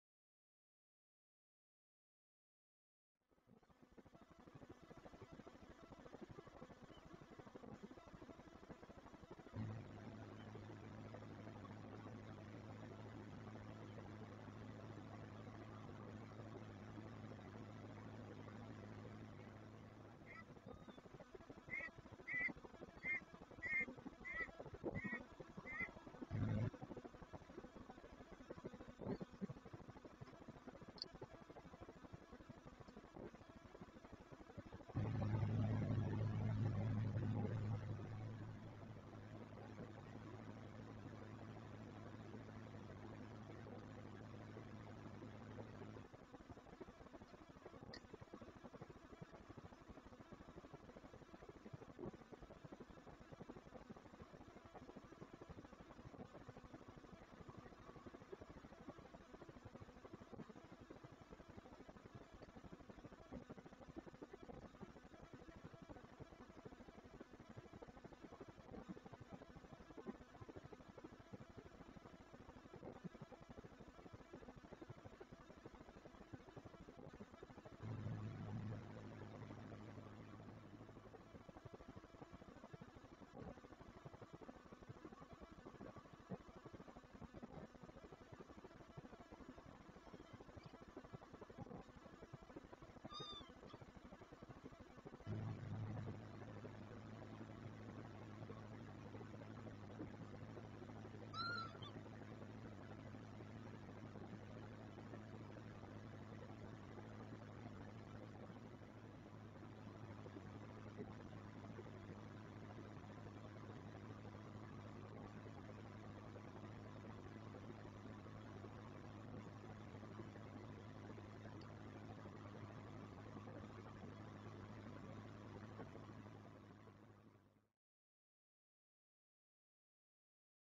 05 05 2020 Plasdras Kievit met 2 jongen
Er is al enige tijd een kievit met twee kuikens rond de plas te zien. De jongen zijn naar insecten op jacht langs het water. De ouders houden de omgeving in de gaten.